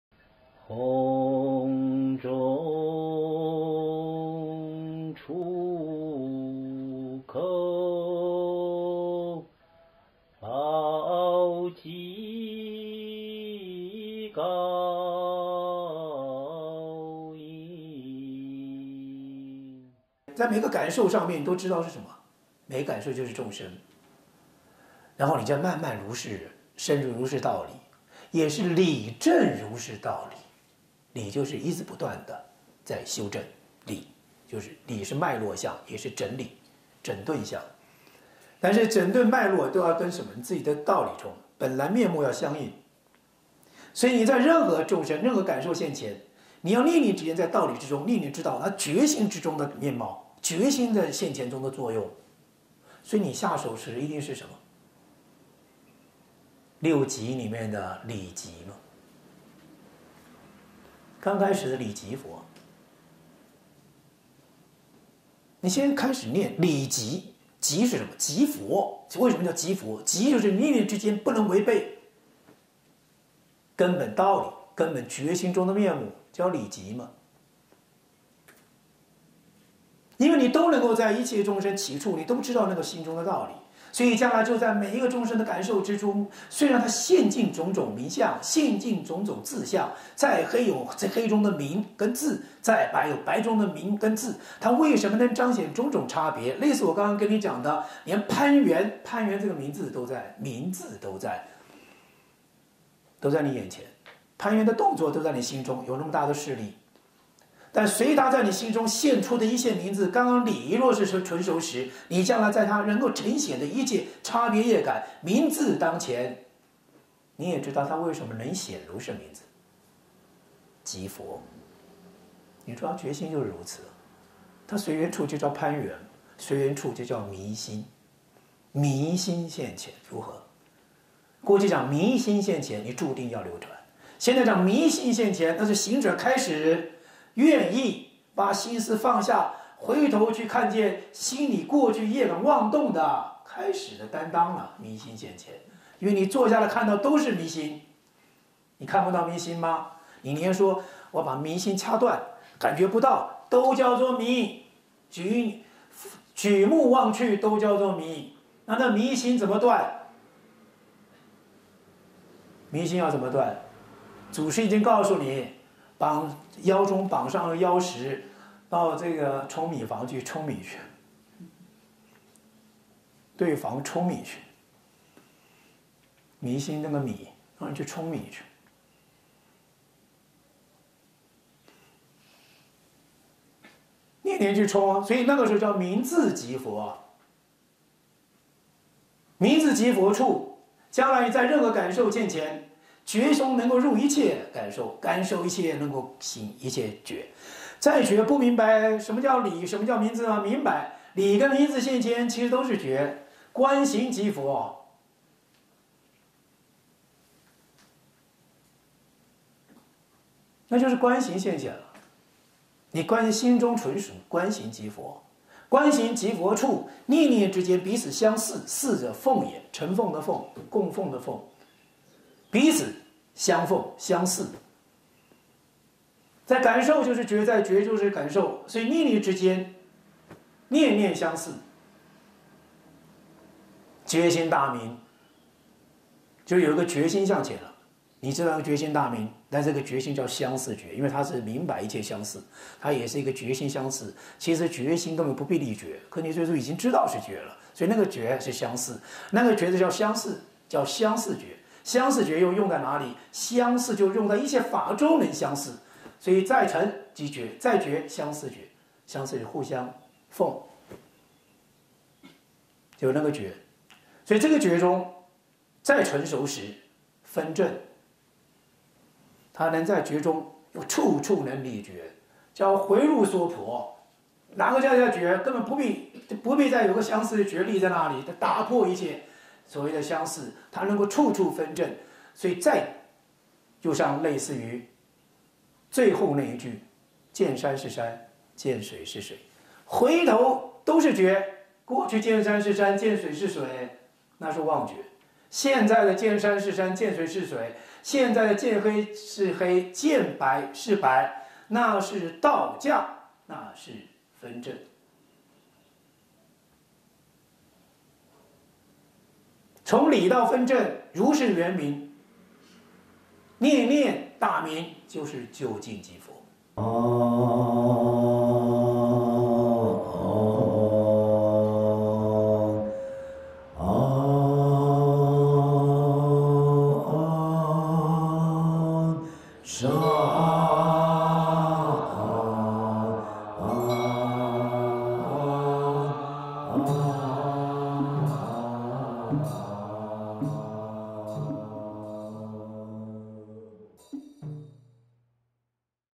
重講《華嚴經義海百門》節錄0083-005講 上自下然和尚宣講
【天台六即佛】

在每個感受上面你都知道是什麼，每個感受就是眾生。然後你在慢慢如是，深入如是道理。也是理證如是道理，理就是一直不斷的在修正，理就是，理是脈絡相也是整理。整頓相，但是整頓脈絡都要跟什麼？你自己的道理中，本來面目要相應，所以你在任何眾生任何感受現前，你要念念之間在道理之中念念知道他覺心之中的面貌，覺心的現前中的作用，所以你下手時一定是什麼？六即裡面的理即嘛。剛開始理即佛，你先開始念理即。即是什麼，即佛，為什麼叫即佛，即就是念念之間不能違背根本道理，根本覺心中的面目叫理即嘛。因為你都能夠在一切眾生起處，你都知道那個心中的道理，所以將來就在每一個眾生的感受之中，雖然他現盡種種名相，現盡種種自相，在黑有黑中的名跟字，在白有白中的名跟字，他為什麼能彰顯種種差別？類似我剛剛跟你講的，連攀緣，攀緣這個名字都在，名字都在，都在你眼前，攀緣的動作都在你心中有那麼大的勢力，但隨他在你心中現出的一切名字，剛剛理若是純熟時，你將來在他能夠成顯的一切差別業感名字當前，你也知道他為什麼能顯如是名字，即佛。你知道覺心就如此啊，他隨緣處就叫攀緣，隨緣處就叫迷心，迷心現前如何？過去講迷心現前你注定要流轉   ，現在講迷心現前那是行者開始願意把心思放下，回頭去看見心裡過去業感妄動的開始的擔當啊，迷心現前。因為你坐下來看的都是迷心，你看不到迷心嘛，你能說我把迷心掐斷，感覺不到都叫做迷，舉目望去都叫做迷，那個迷心怎麼斷？迷心要怎麼斷？祖師已經告訴你腰中綁上腰石。到這個舂米房去舂米去，碓房舂米去，迷心那個米，讓你去舂米去，念念去舂啊，所以那個就叫名字即佛啊，名字即佛處，將來你在任何感受現前，覺中能夠入一切感受，感受能夠醒一切覺，在覺不明白什麼叫理，什麼叫名字嗎？明白，理的名字現前其實都是覺，觀行即佛，那就是觀行現前了，你觀心中純熟，觀行即佛，觀行即佛處念念之間彼此相似，似者奉也，呈奉的奉，供奉的奉。彼此相奉相似，在感受就是覺，在覺就是感受，所以在念念之間，念念相似，覺心大明。就有個覺心現前了，你知道是覺心大明，但這個覺心叫相似覺，因為他是明白一切相似，他也是跟覺心相似，其實覺心跟本不必立覺，可你這時已經知道是覺了，所以那個覺是相似，那個覺叫相似叫相似覺，相似覺又用在哪裡，相似就用在一切法中能相似，所以在塵即覺，在覺相似覺，相似就互相奉，就是那個覺，所以這個覺中在成熟時，分證，他能在覺中處處能立覺，叫回入娑婆，哪裡再說覺？根本不必，不必再由相似覺立在那裡，他打破一切所謂的相似，他能夠處處分證，所以在，就像類似於，最後那一句。見山是山，見水是水。回頭都是覺，過去見山是山，見水是水，那是妄覺。現在的見山是山，見水是水，現在的見黑是黑見白是白，那是倒駕那是分證。
從理到分證如是圓明，念念大明就是究竟即佛。

節錄於上自下然和尚重講《華嚴經義海百門》第0083